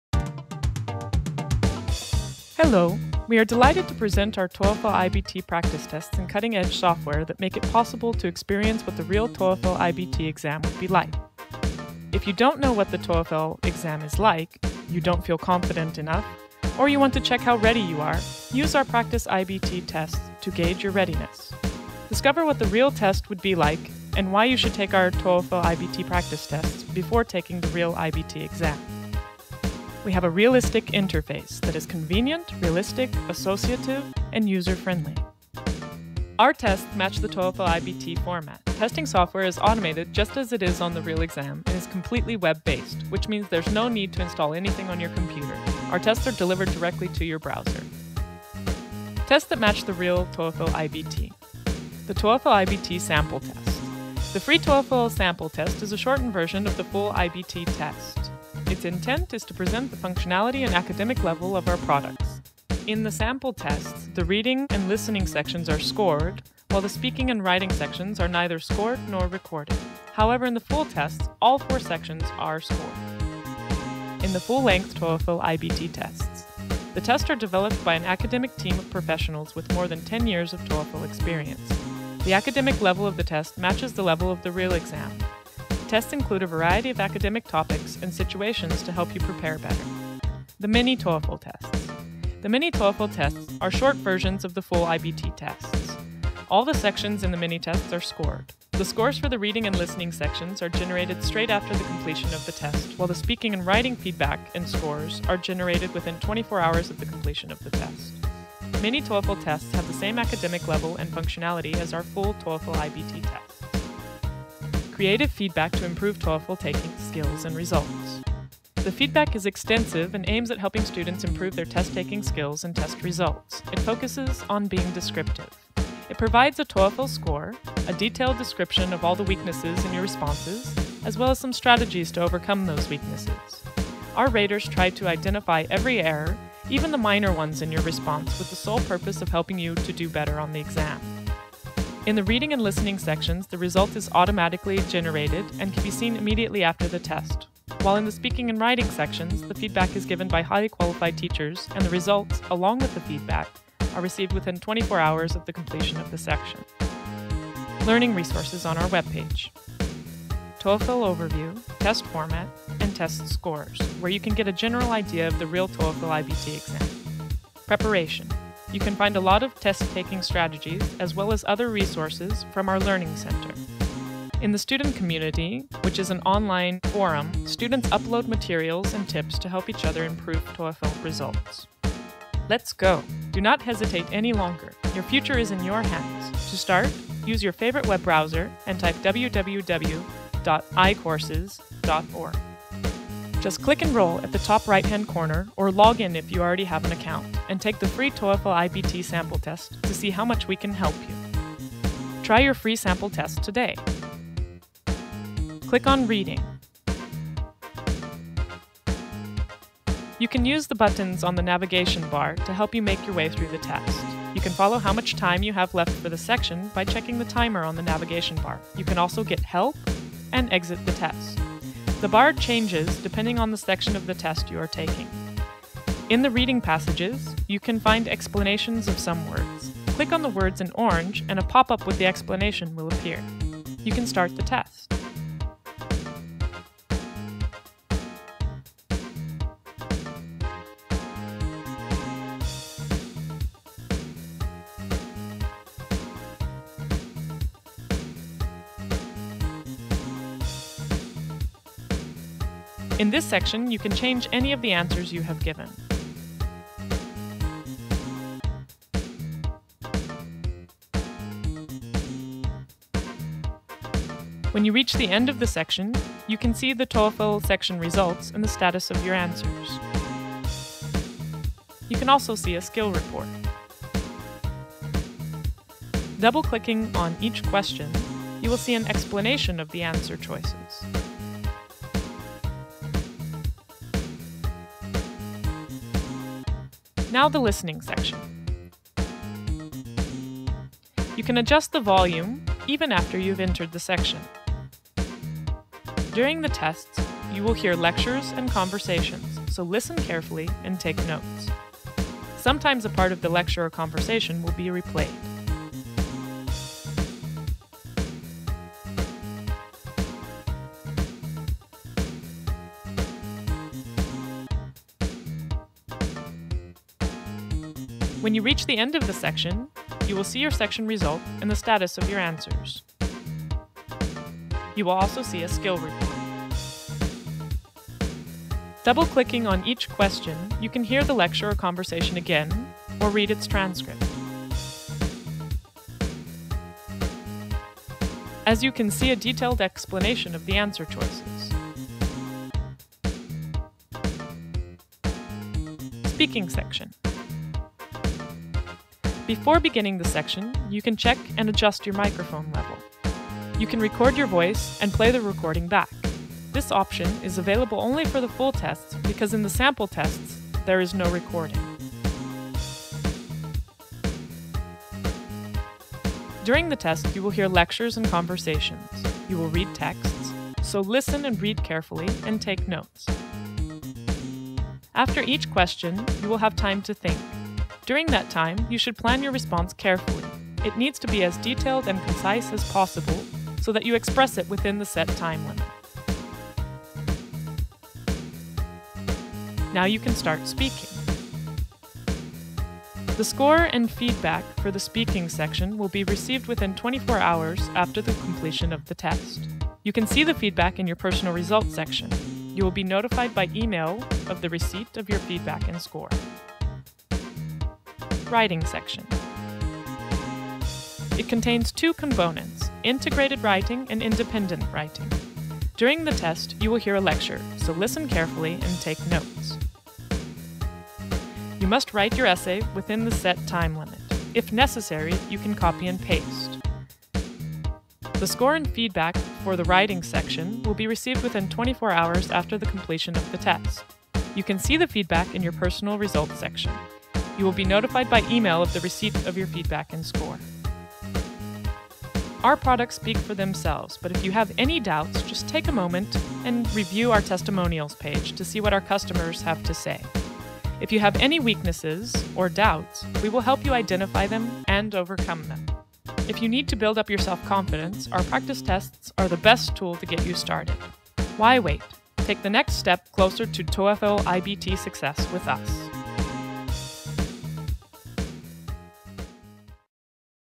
TOEFL iBT Testing System Tour
A detailed presentation on how to use i-Courses' TOEFL testing software. delivers realistic and best quality TOEFL iBT Practice tests and learning materials.